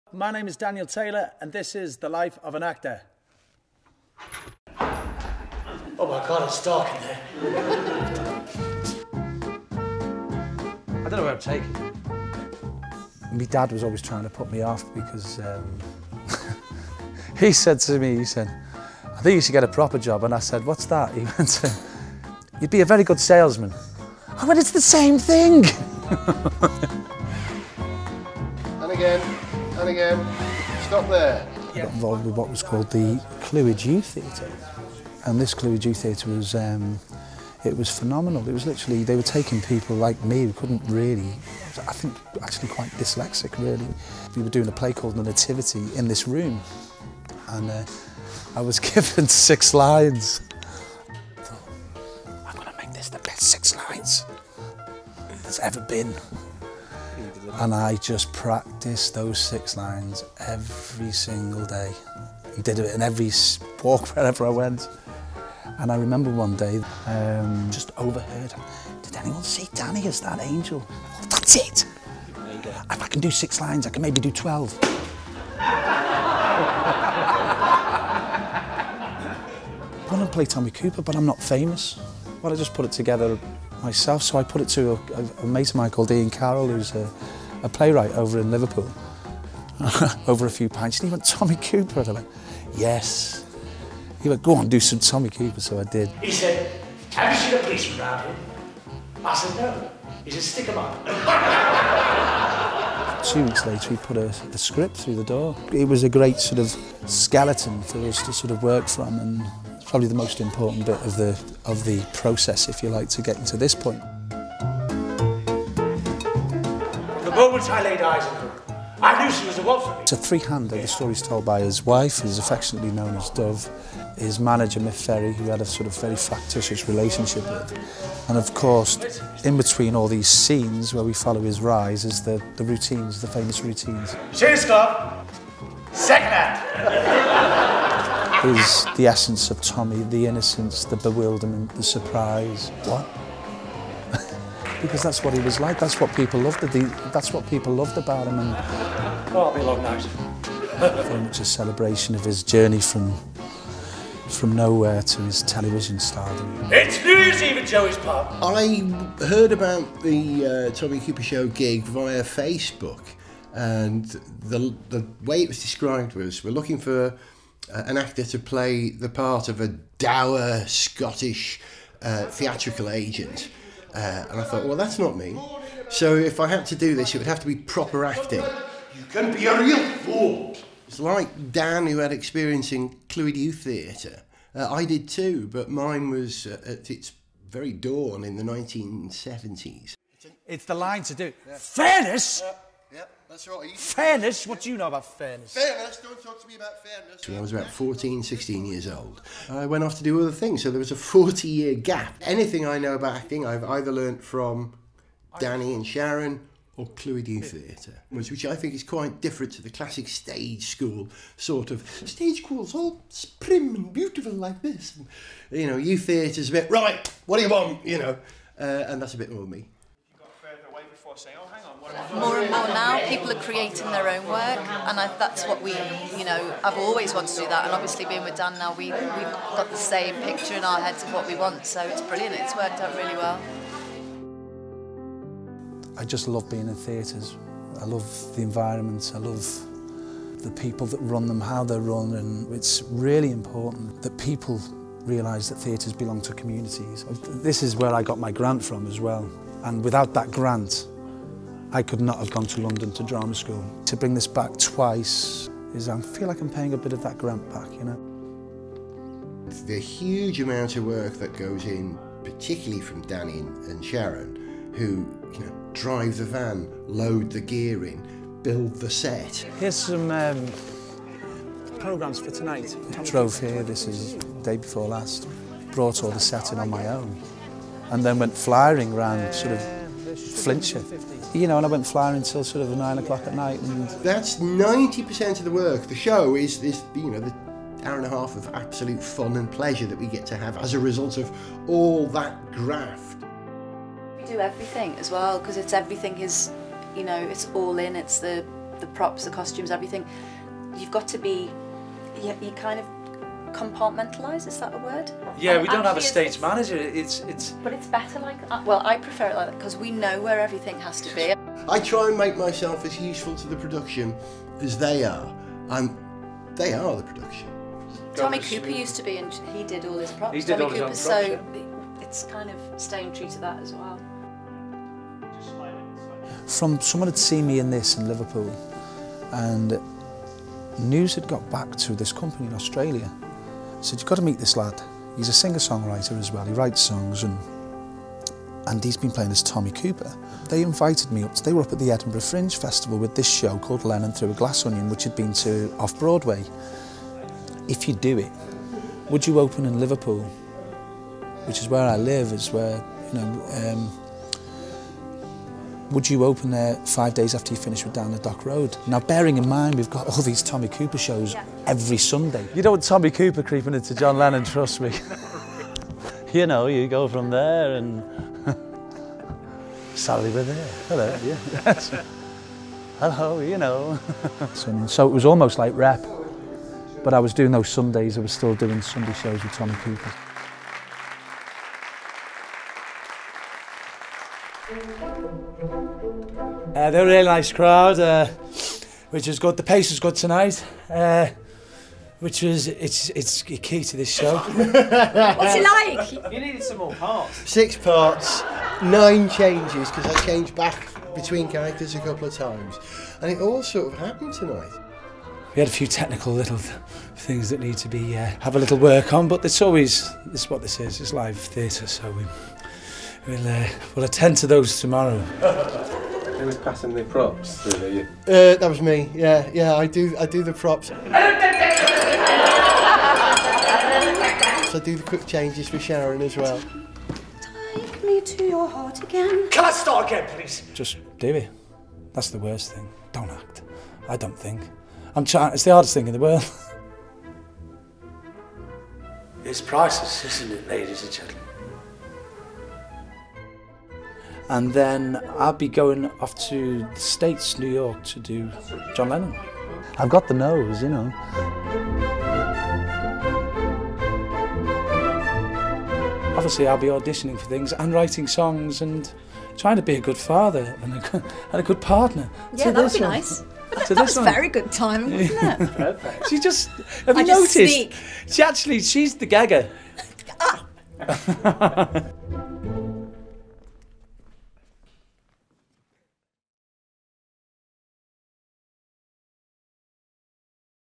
Life of An Actor - with Daniel Taylor
Life of an Actor is the latest in our series of inspirational documentaries profiling creative people.

Daniel Taylor has been a professional actor for more than 20 years, treading the boards in West End musicals like Blood Brothers, Bleasdale-penned plays like Down The Dock Road and more recently portraying well-known figures like John Lennon and Tommy Cooper.